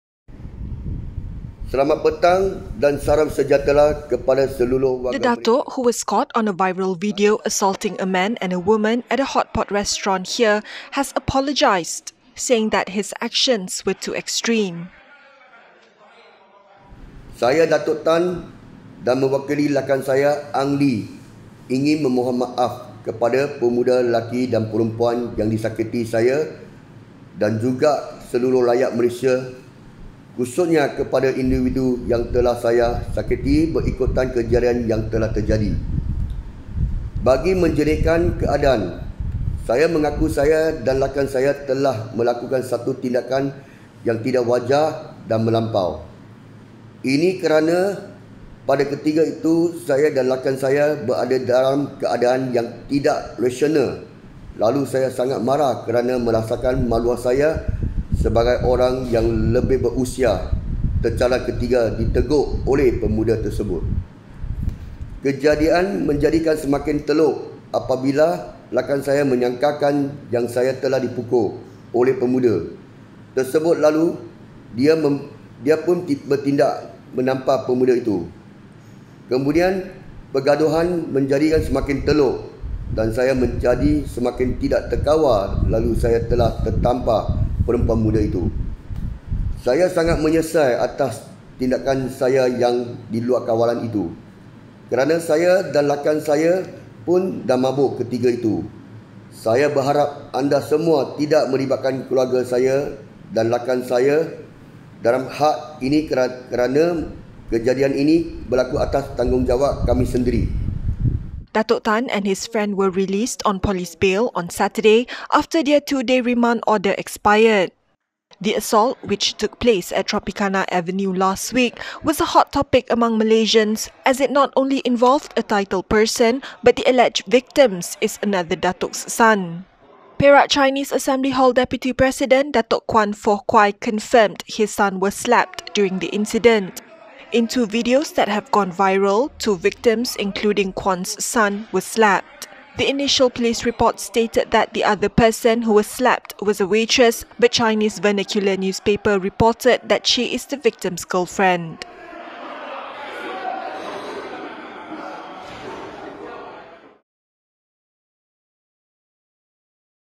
Datuk in viral hotpot slapping video apologises
The Datuk who was caught on a viral video assaulting a man and a woman at a hotpot restaurant here has apologised, saying that his actions were too extreme.

The man who identified himself as Datuk Tan said that in a video message that went viral on Monday (Jan 18) apologising also on behalf of his friend, whom he called Ang Lee, to the man and woman, as well as Malaysians for his action.